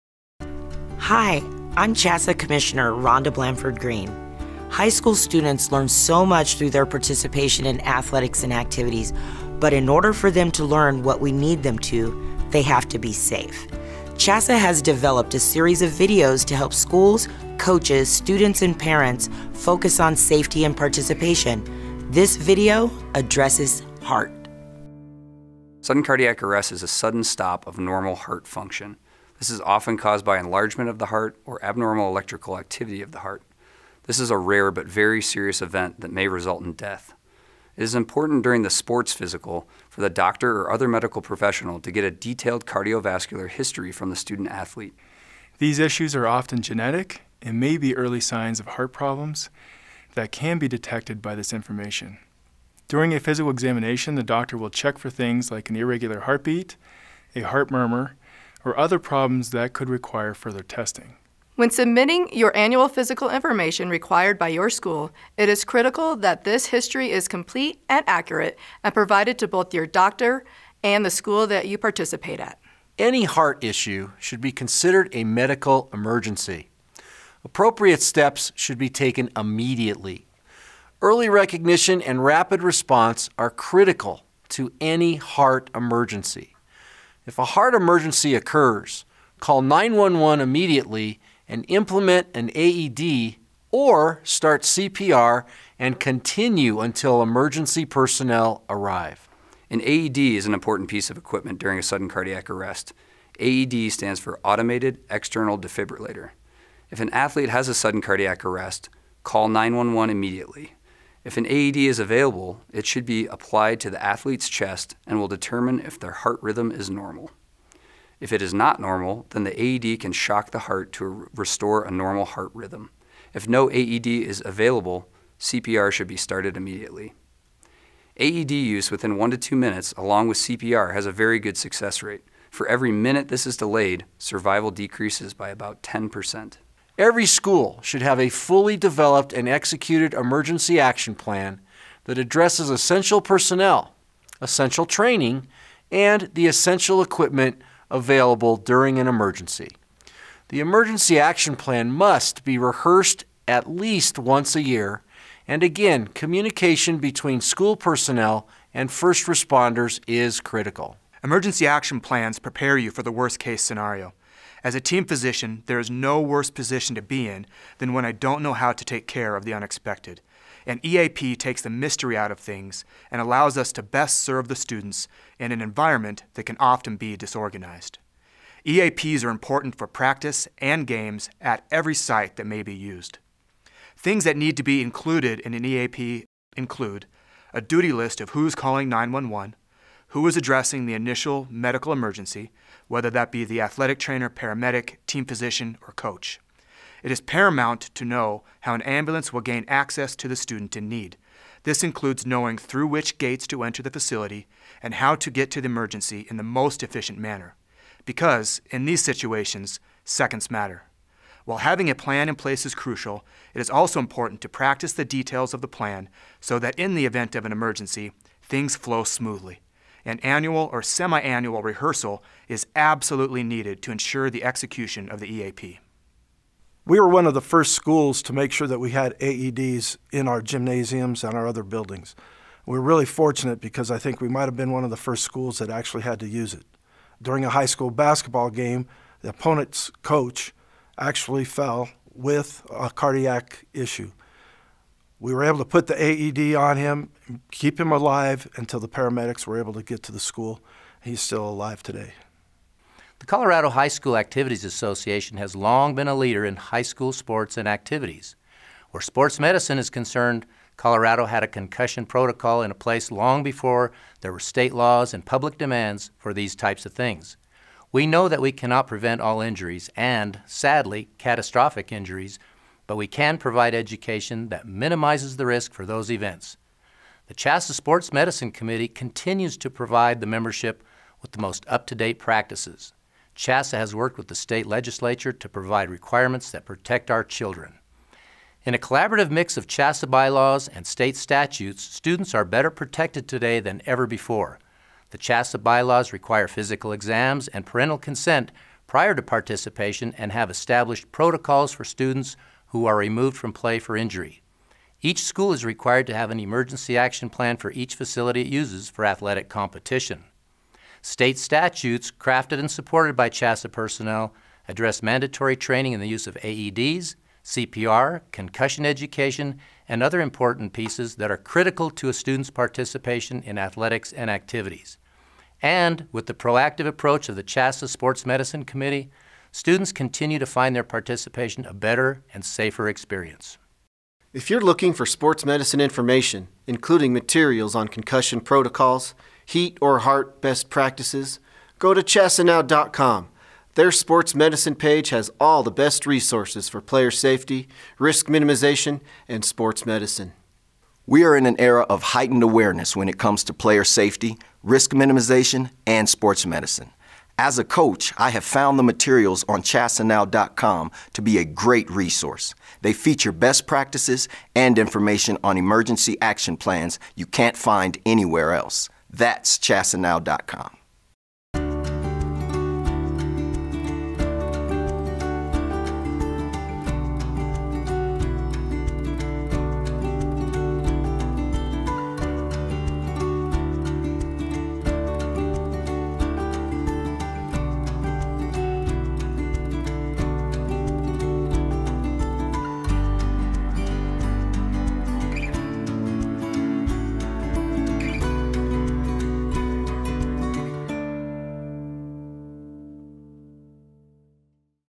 Head, Heart & Heat: Cardiac arrest and using AEDs
Experts from around Colorado discuss cardiac arrest, including prevention and awareness, and how to save a life with an AED. Also includes information on emergency action plans. (Video: CHSAA & Sports Medicine Advisory Committee.)